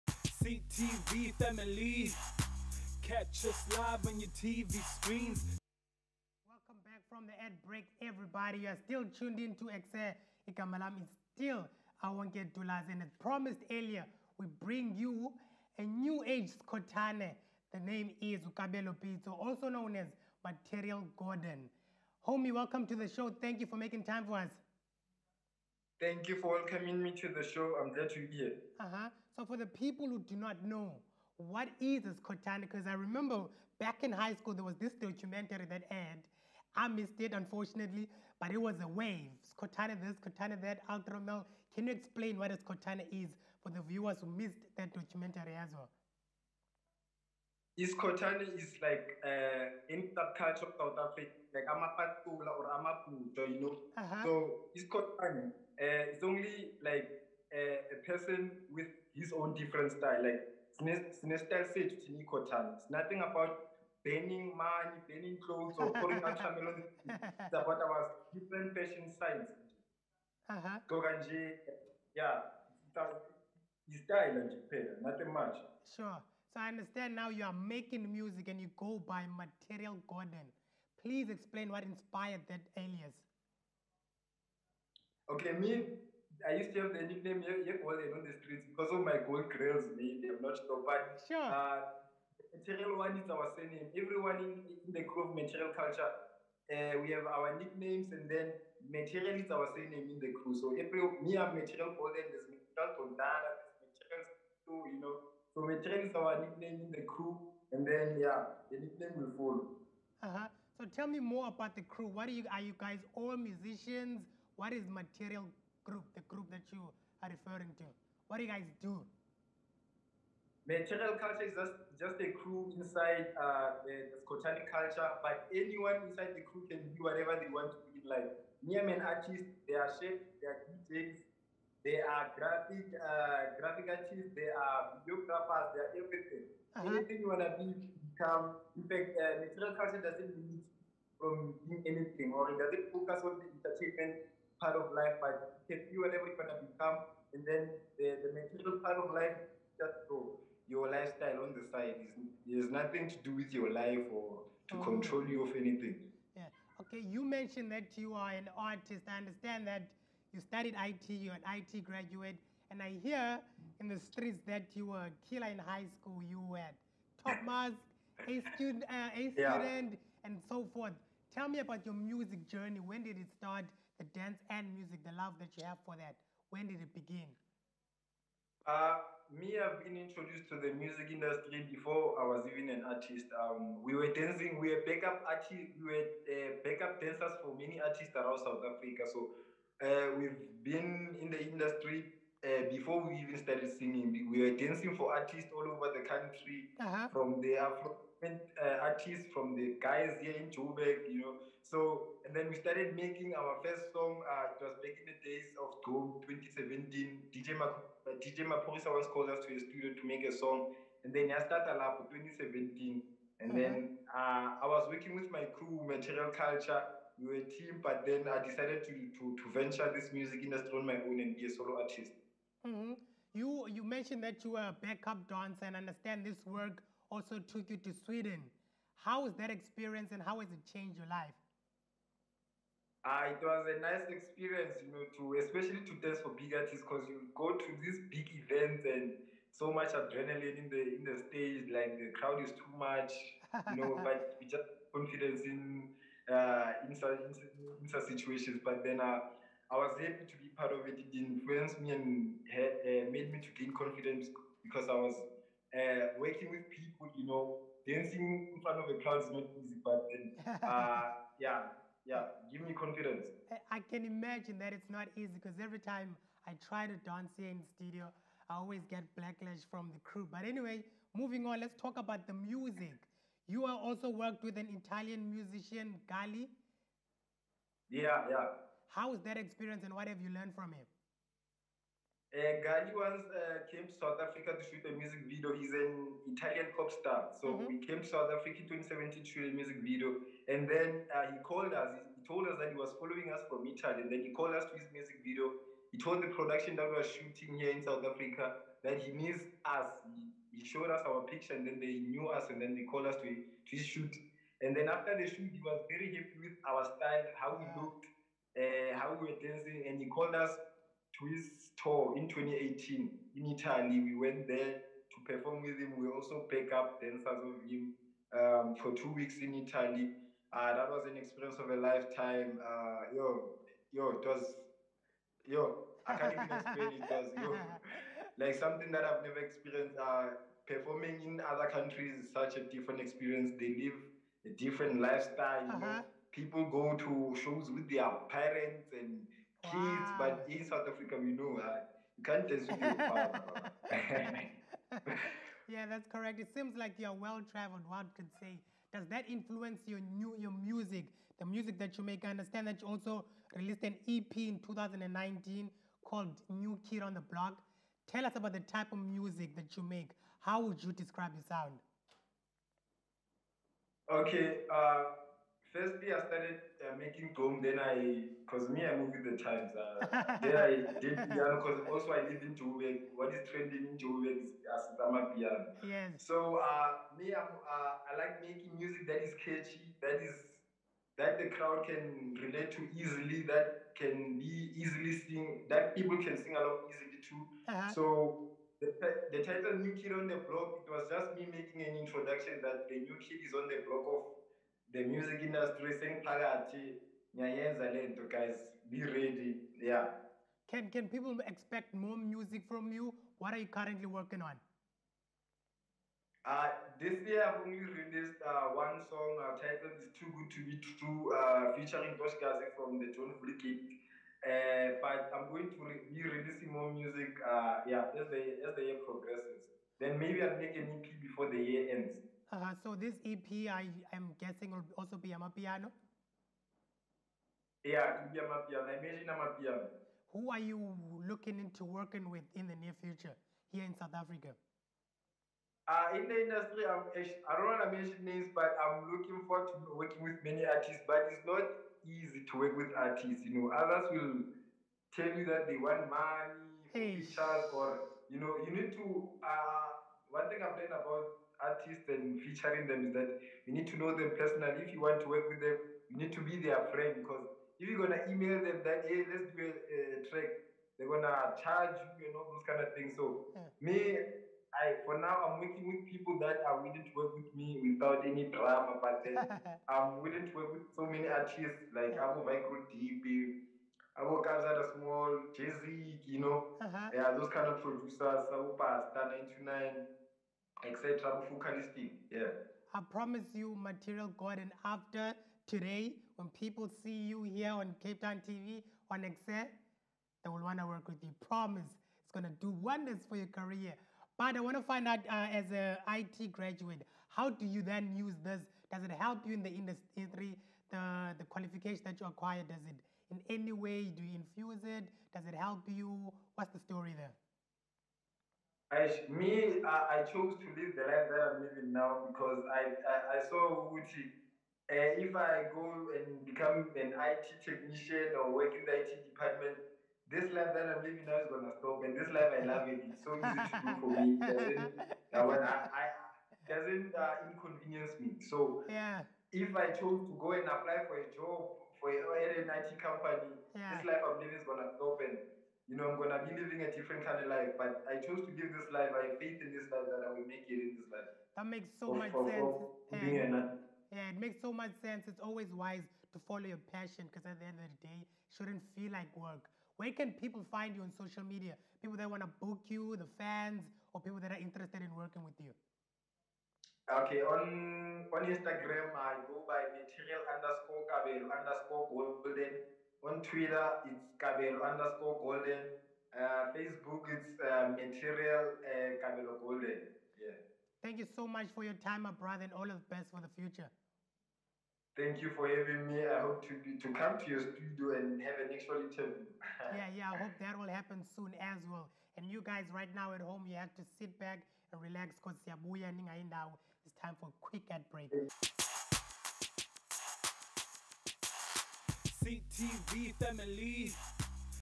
Ekse LIVE (Material Golden)
Meet the New age Skhothane, cava the style, cave the swag, cava the vibe - get to know him better in this exclusive interview. Ladies and gents, stand up for Material Golden!